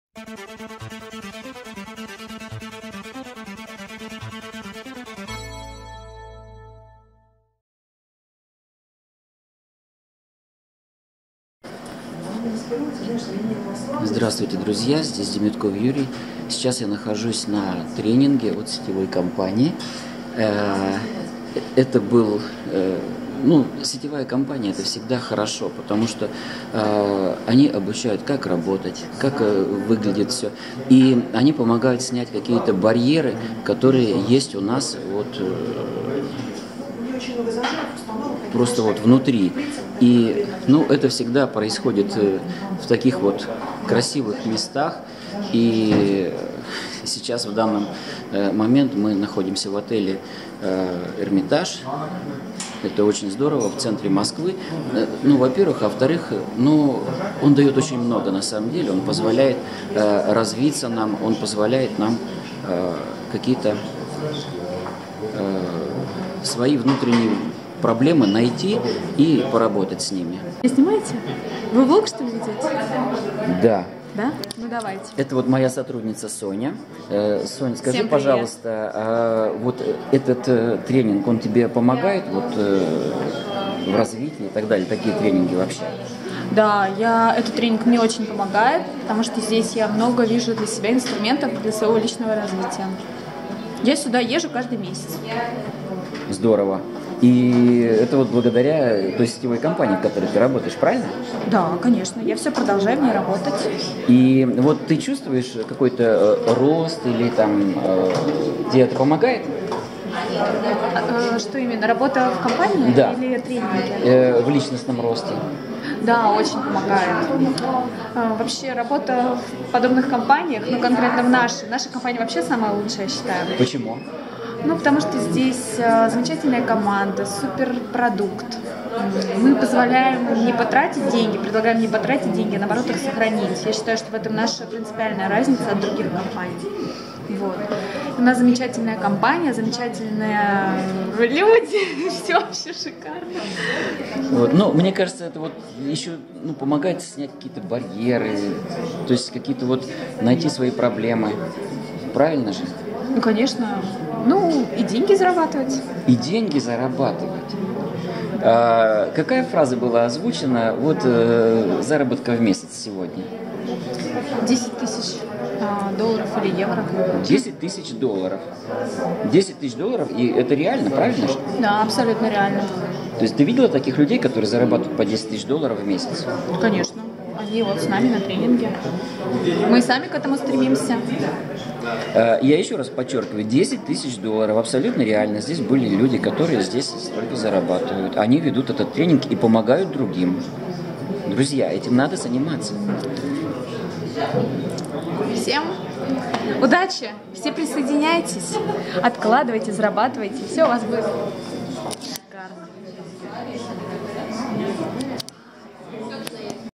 Репортаж с тренинга
Короткий репортаж с тренинга швейцарской сетевой компании - крупнейшего международного маклера по социально-накопительным программам.